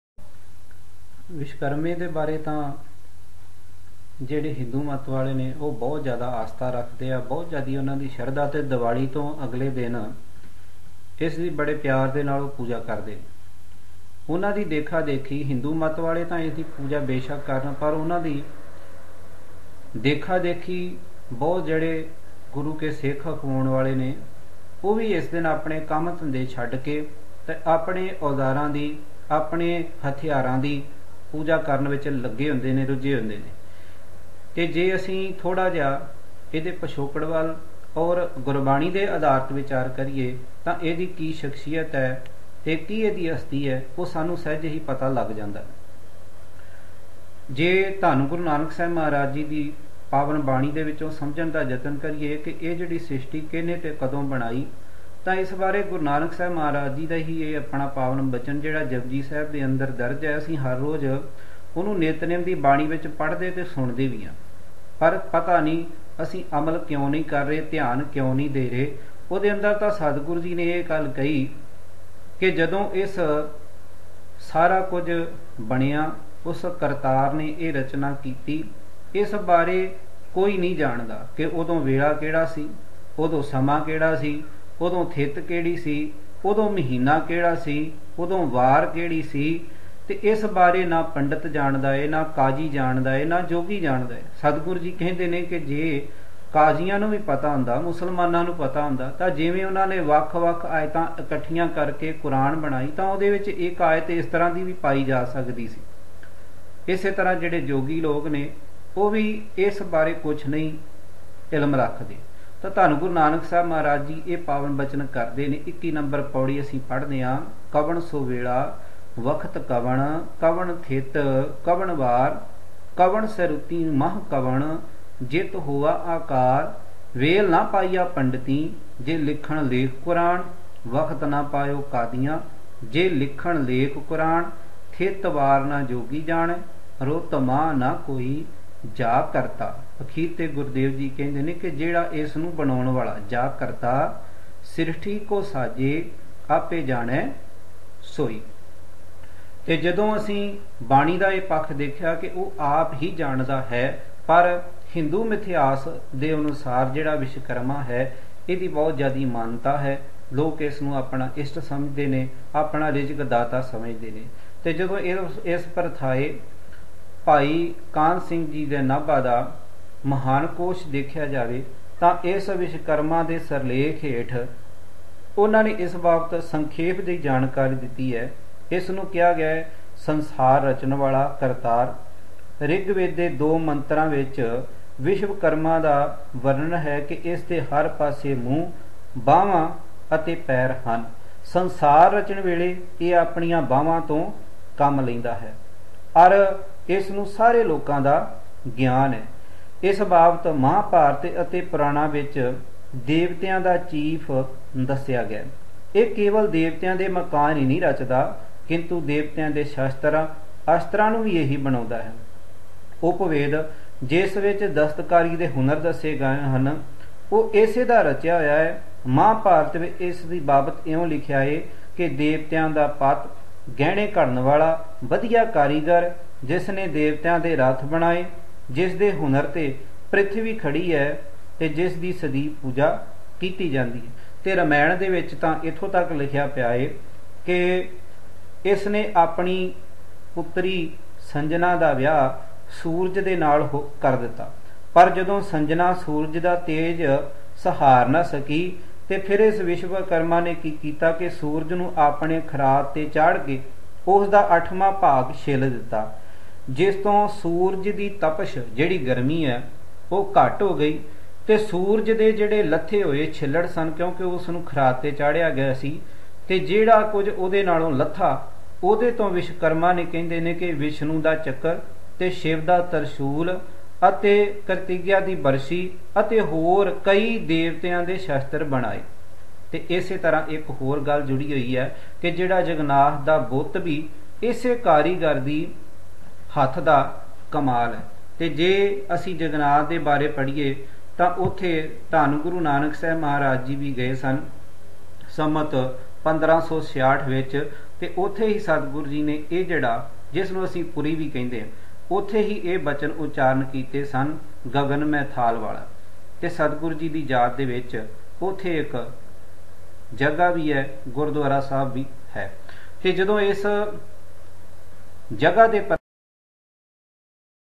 VishavKarma day-ਵਿਸ਼ਵਕਰਮਾ ਬਾਰੇ Bhai Gurjant Singh Rupowali
viahav karma day
ਵਿਸ਼ਵਕਰਮਾ ਬਾਰੇ ਸੰਖੇਪ ਵੀਚਾਰ
ਭਾਈ ਗੁਰਜੰਟ ਸਿੰਘ 'ਰੂਪੋਵਾਲੀ'